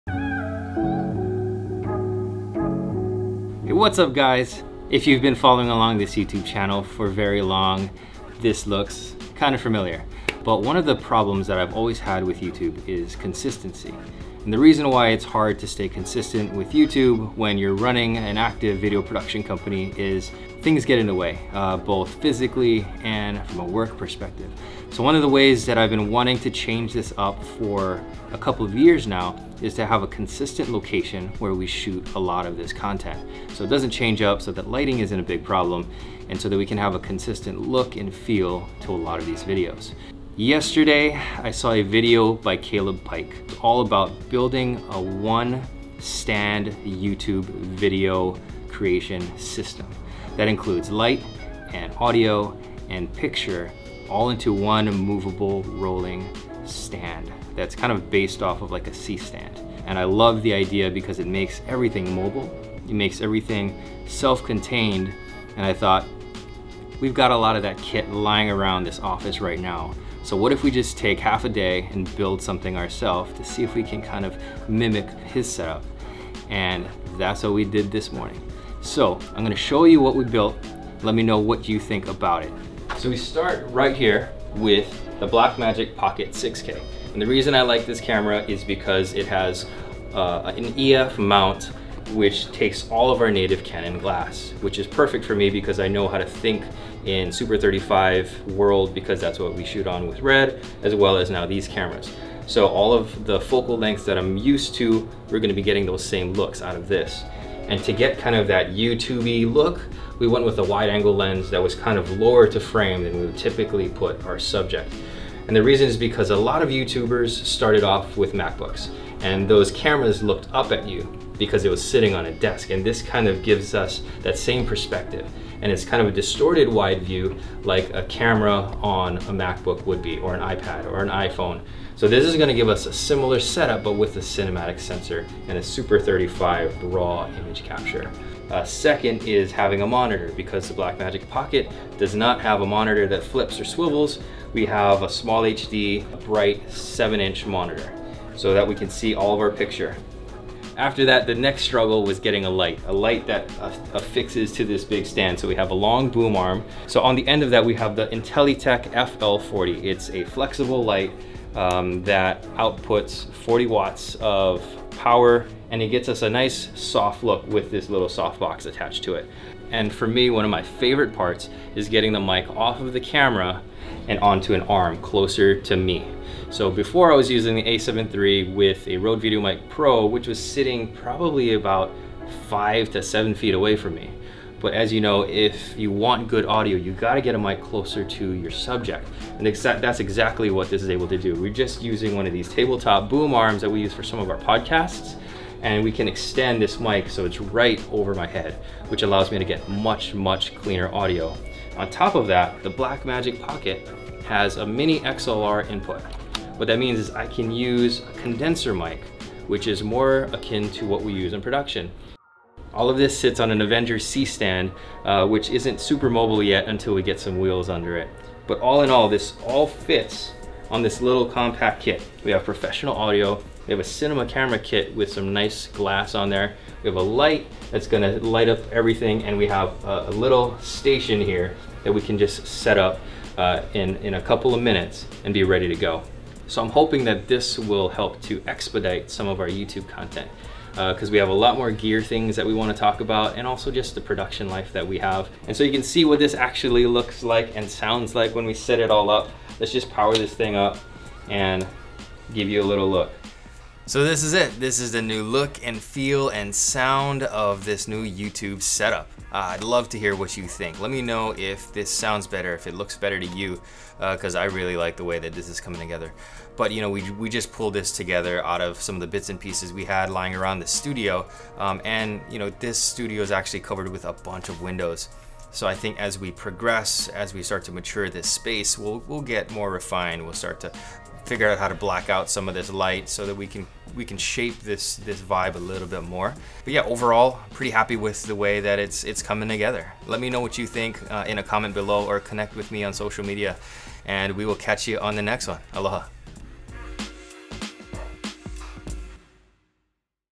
How to Build a YouTube Studio with one stand!
When it comes to making YouTube videos, one of the biggest problems that I face, is consistency. Keeping up with YouTube content while running an active video production company can be crazy difficult… things always seem to get in the way, both physically and work-wise.
One of the things I’ve been wanting to do for a while is set aside an area of the studio to build a dedicated YouTube space that would eliminate the need to assemble a kit every time we wanted to make a new video. Low and behold Caleb Pike’s video “Full Youtube Studio in One Stand” popped into my feed and I knew that’s exactly what we needed to build. So we gathered up a bunch of gear laying around the shop and put together our own version of Caleb’s kit.

Let me know what you think about the clean look and sound and if you have any tips for making it better!

Aloha

GEAR:

Camera: BlackMagic Pocket Cinema Camera 6k
Lens: Canon 17-55
Monitor: SmallHD 702 Bright
Shotgun Condenser Mic: Sennheiser ME64
Clamps: Manfrotto Super Clamp
Arms: Wooden Camera Arm
Mic Stand: Neewer Adjustable Mic Stand

Other Camera
Vlog Camera: Sony A73
Vlog Lens: Tamron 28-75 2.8
Vlog Detail Lens: Sigma 24/1.4
Sigma EF Adapter